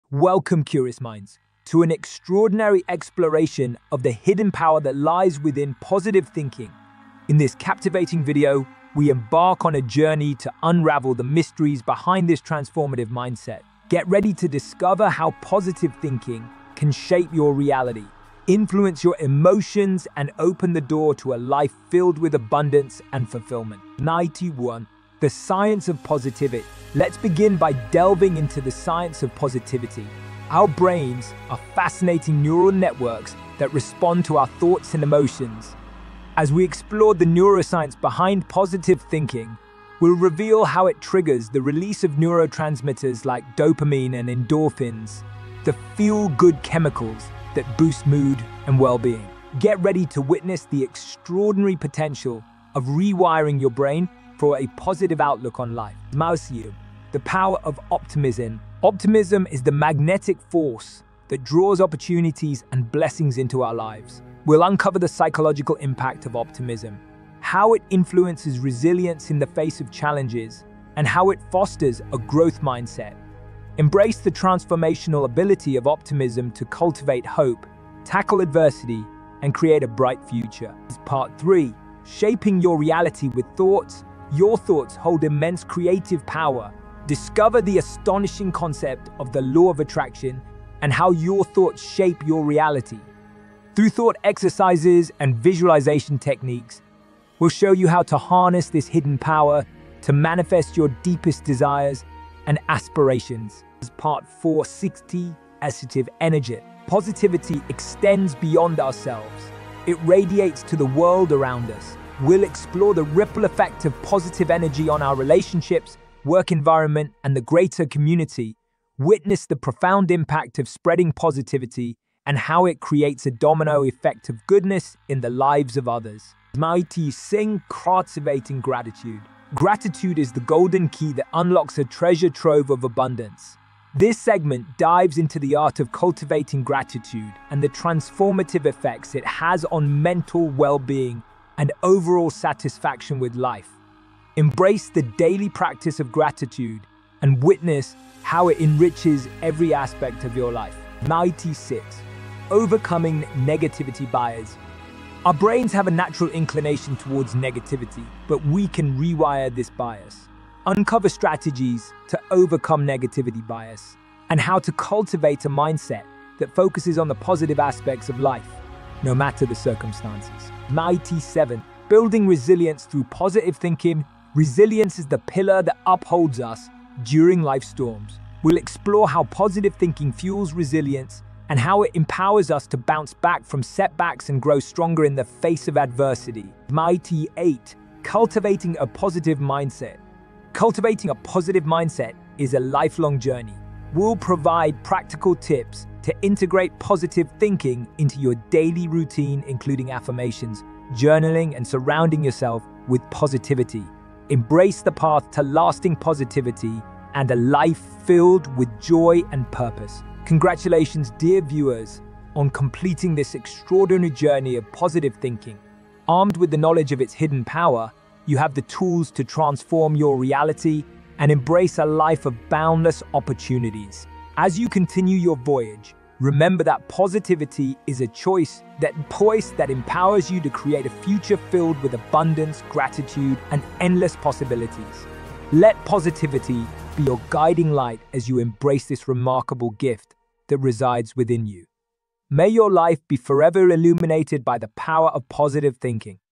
The Hidden Power of Positive Thinking
Welcome to an extraordinary exploration of the hidden power that lies within positive thinking. In this captivating video, we embark on a journey to unravel the mysteries behind this transformative mindset. Prepare to discover how positive thinking can shape your reality, influence your emotions, and open the door to a life filled with abundance and fulfillment.

Positive thinking isn't just wishful optimism; it's rooted in science. We'll delve into the neuroscience behind positive thinking, exploring how it triggers the release of neurotransmitters like dopamine and endorphins, the feel-good chemicals that boost mood and well-being.

Optimism is the magnetic force that draws opportunities and blessings into our lives. We'll explore the psychological impact of optimism, how it influences resilience in the face of challenges, and how it fosters a growth mindset.

Your thoughts hold immense creative power. Through thought exercises and visualization techniques, we'll show you how to harness this hidden power to manifest your deepest desires and aspirations.

Positivity extends beyond ourselves; it radiates to the world around us. We'll explore the ripple effect of positive energy on our relationships, work environment, and the greater community.

Limiting beliefs can cloud our perception of self. It's time to challenge and overcome these barriers that hinder our growth. We'll explore strategies to replace limiting beliefs with empowering affirmations, unlocking the doors to new possibilities.

Authenticity is the cornerstone of self-discovery. This segment celebrates the beauty of embracing your true self, unapologetically. We'll explore the courage it takes to shed societal expectations and unlock the liberation that comes with living authentically.

The inner journey can be both enlightening and challenging. Embrace the power of mindfulness, as it illuminates the shadows within and empowers us to grow and evolve into our highest selves.

Congratulations, dear explorers, on completing this transformative journey of self-discovery. As you embrace your true potential, may you continue to embark on a lifelong quest of growth, compassion, and authenticity. Remember that self-discovery is an ongoing voyage, and each step forward brings you closer to a life of purpose and fulfillment. Embrace the power that resides within you and allow it to illuminate your path towards self-empowerment and genuine happiness. May your journey be filled with endless wonders and the realization of your true potential.